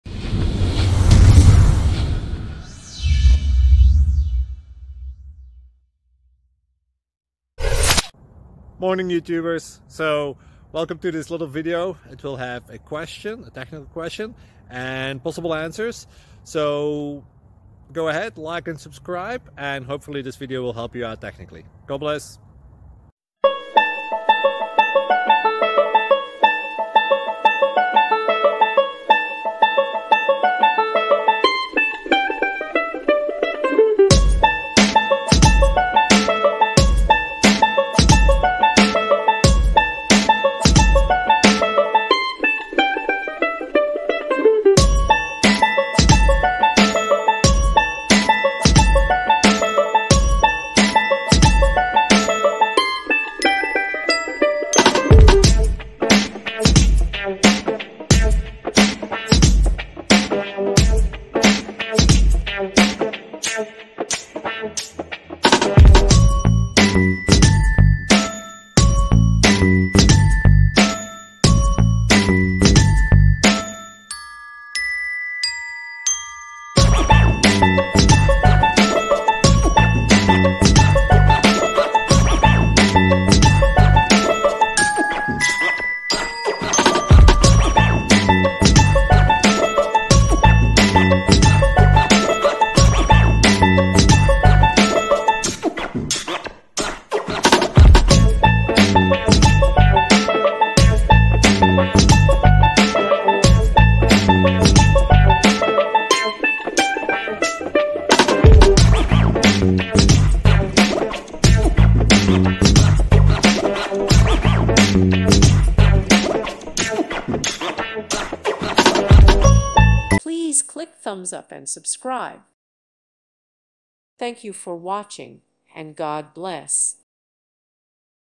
Code Review: PHP framework building: System Class (reads configuration .ini file)
With thanks & praise to God, and with thanks to the many people who have made this project possible! You are responsible for your own actions.